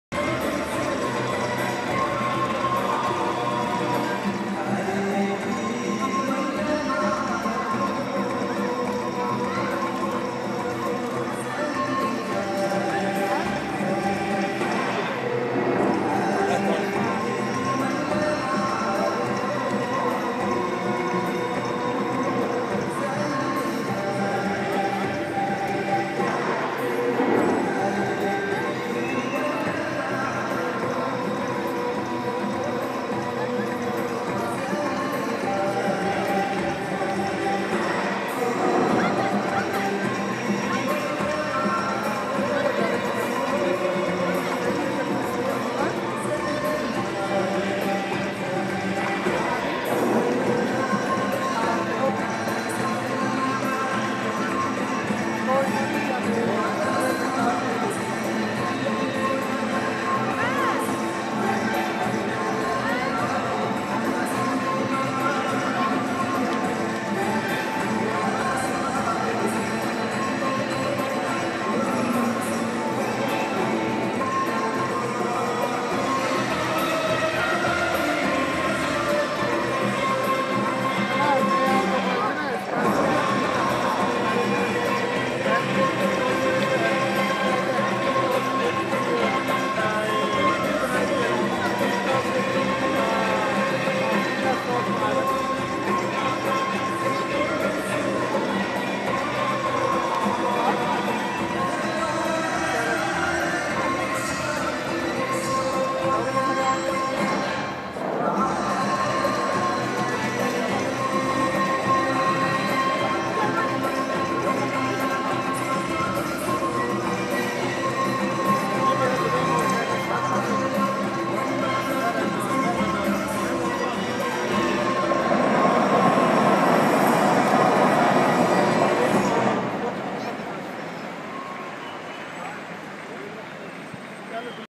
Water Fountain Dance At World's Tallest Building In Dubai - IMRAN™
Dancing Fountains & Music At Foot Of World's Tallest Building In Dubai. Handheld iPhone 4S Video